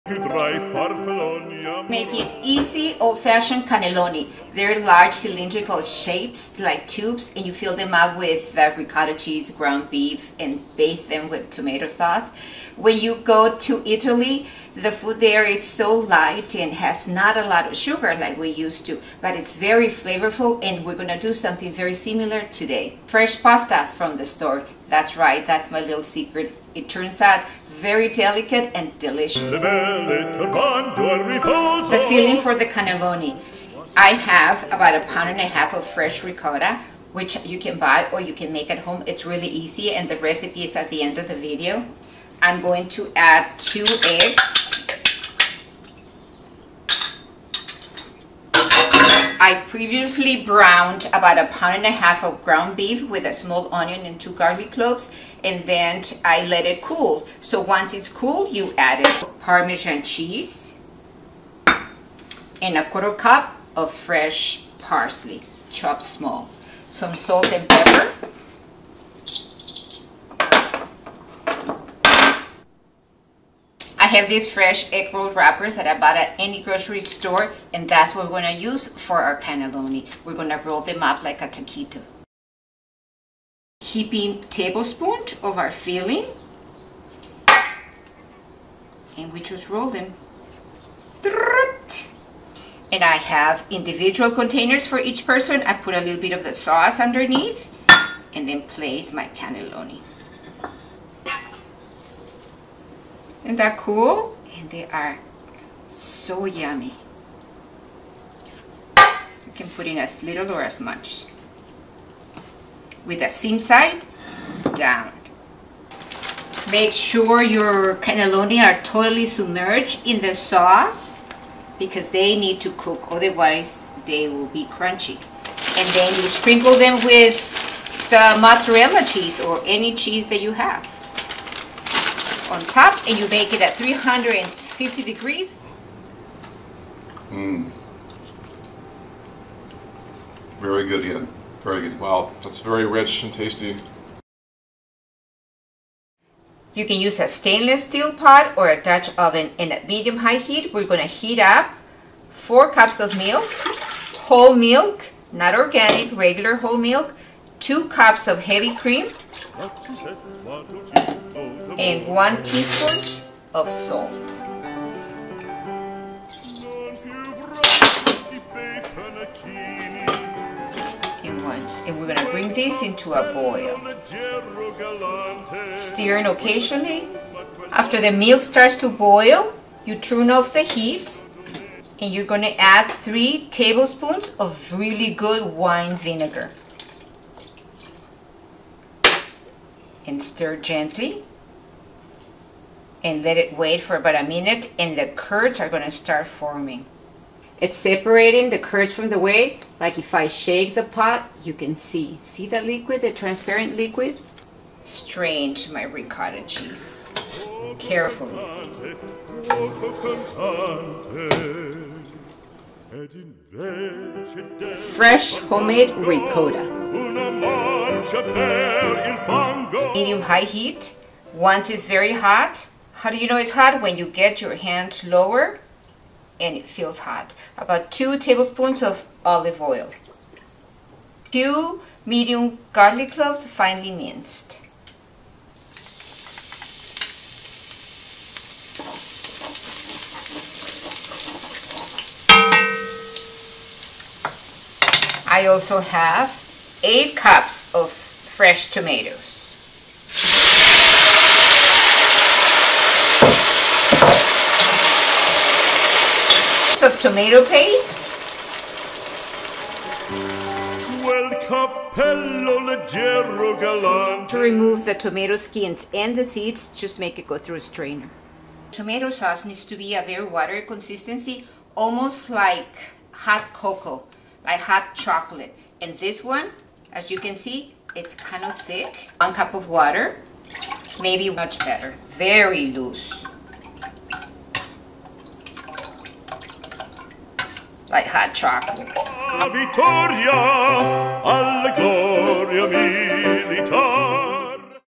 Cannelloni
This old fashion Italian dish is faster to make with fresh pasta from most stores (egg roll wrappers). The creamy ricotta cheese is homemade (super fast) and the tomato sauce is light & flavorful.